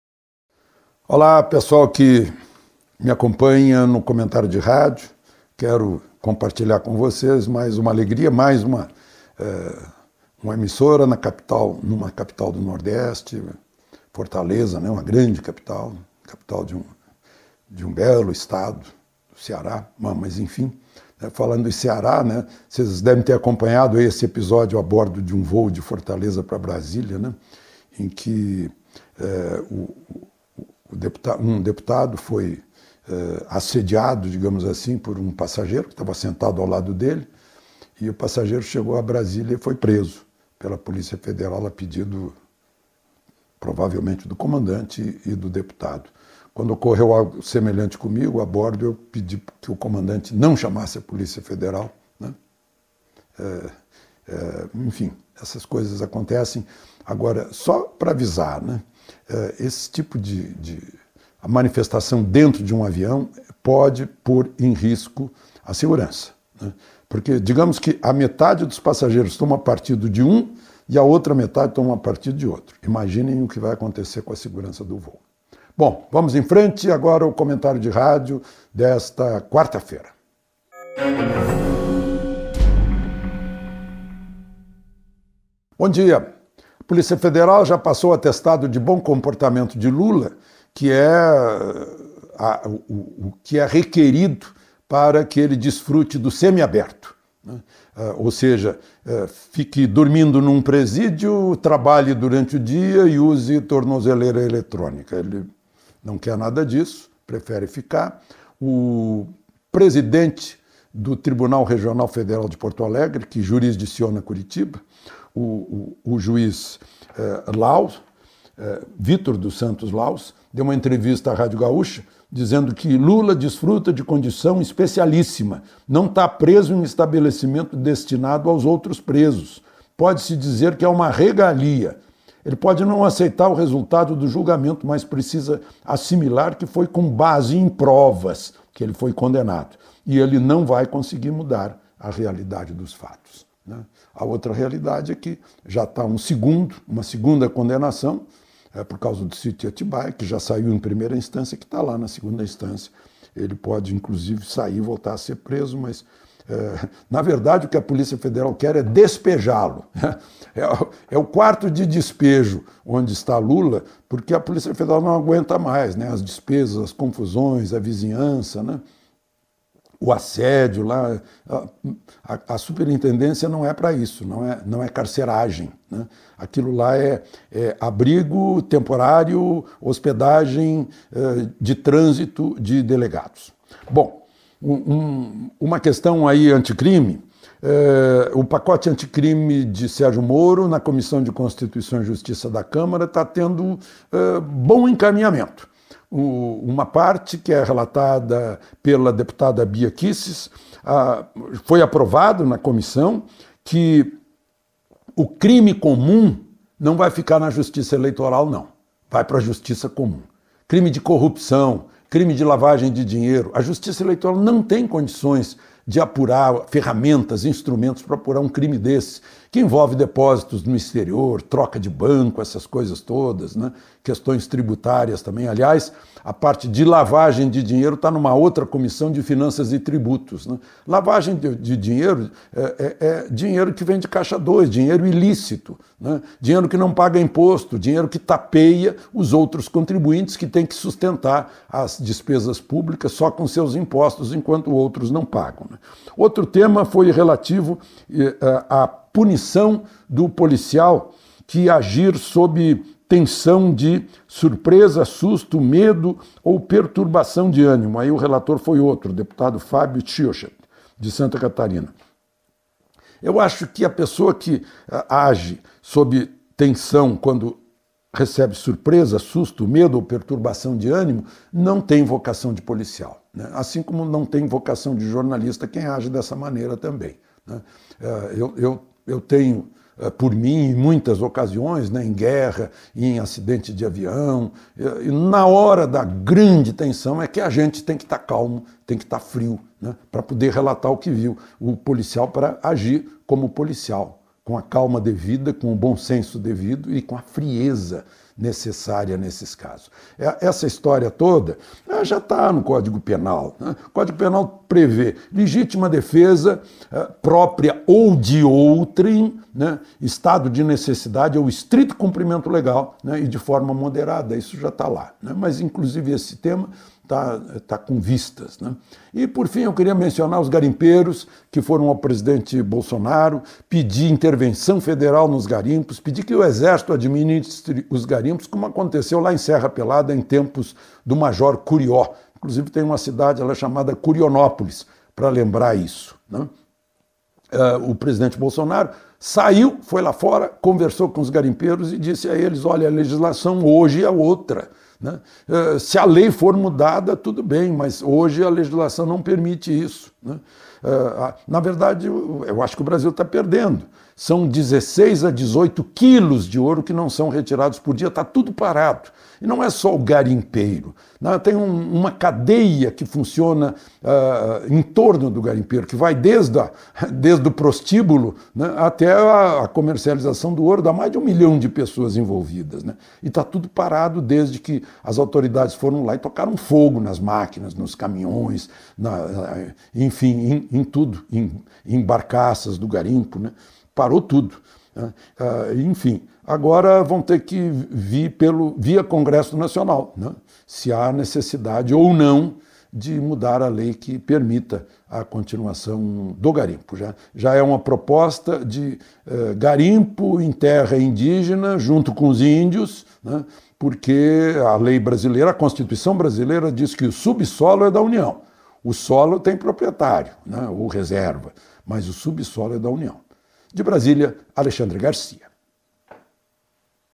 O difícil despejo de Lula da regalia federal
Meu comentario desta quarta-feira em 300 rádios